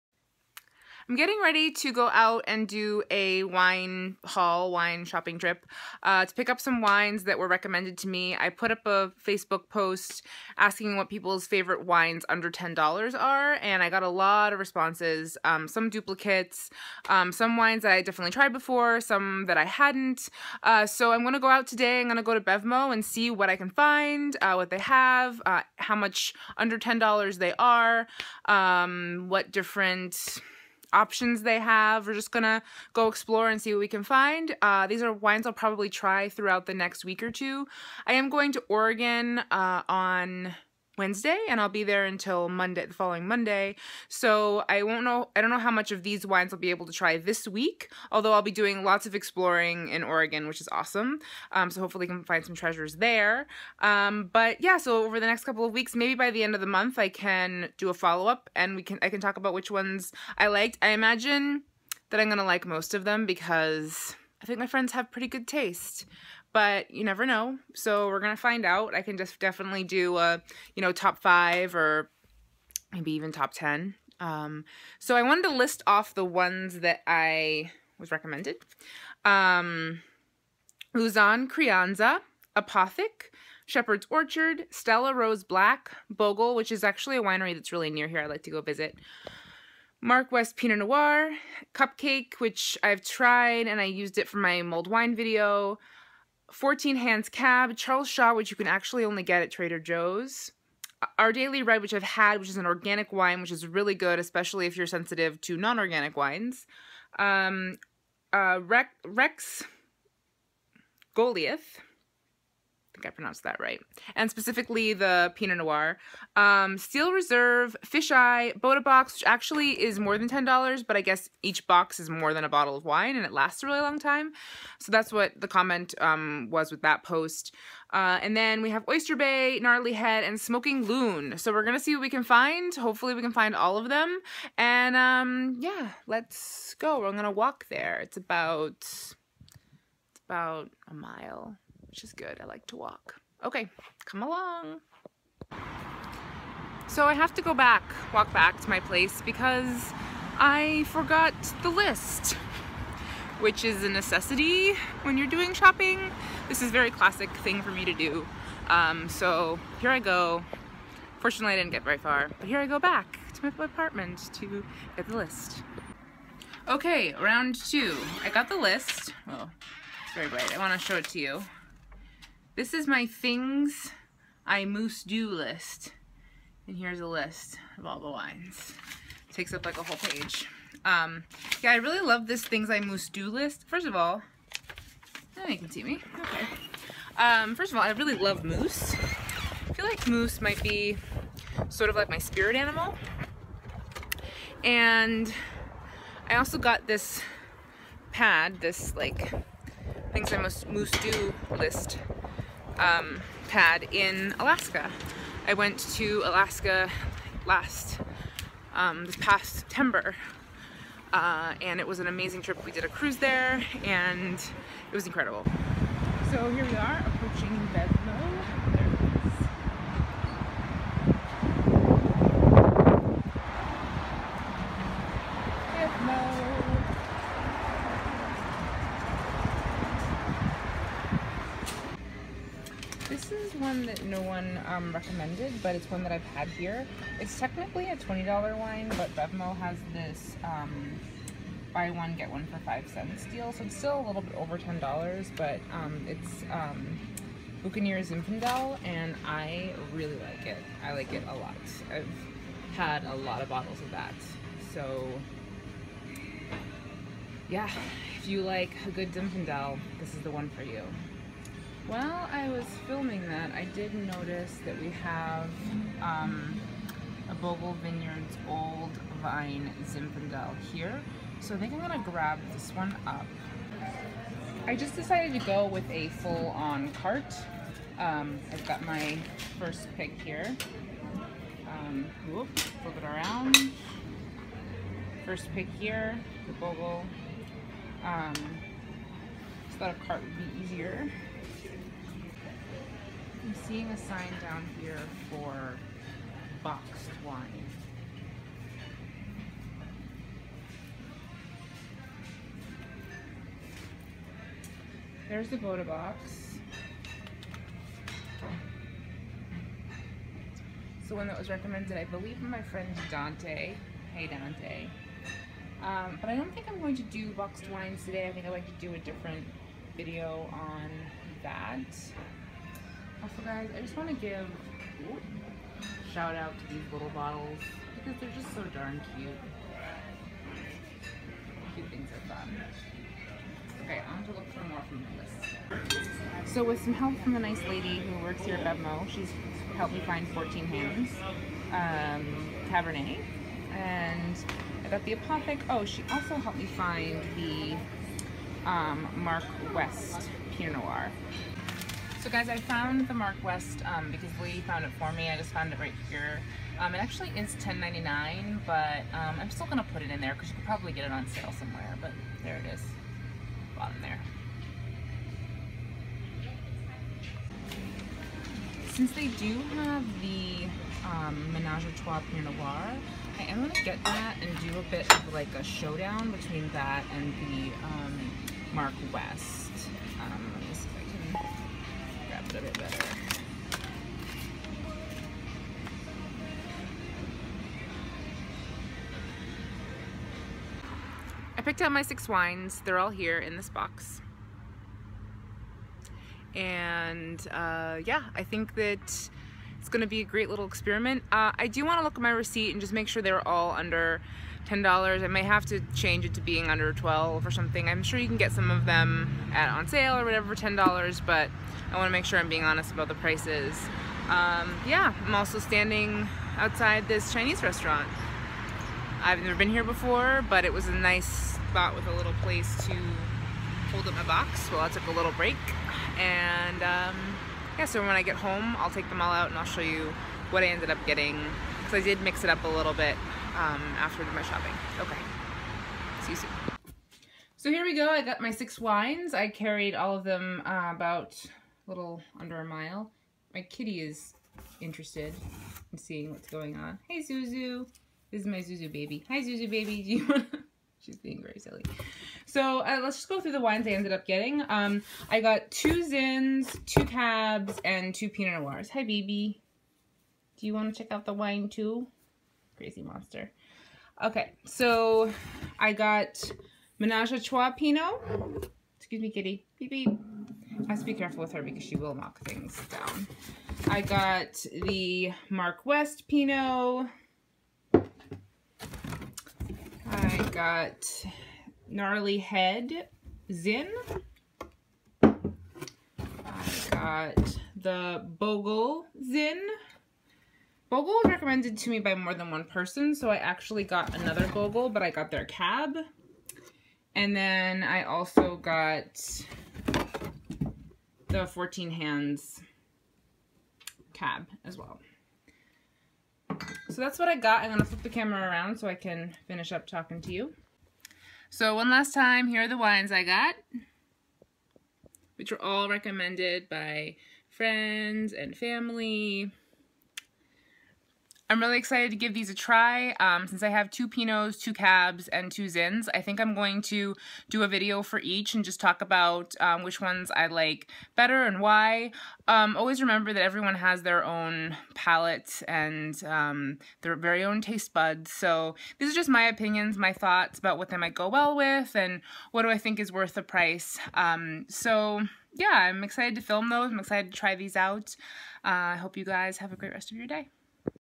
Wine Haul | $8-$12 wines!
Come along with me while I shop for wines recommended by friends and family in the $10-$15 range.

Below is the list of recommend wines

Charles Shaw - Trader Joe's
Our Daily Red
Steel Reserve
Fish Eye
Bota-box
Menage a Trois

Midori Harris
3145 Geary Blvd. #314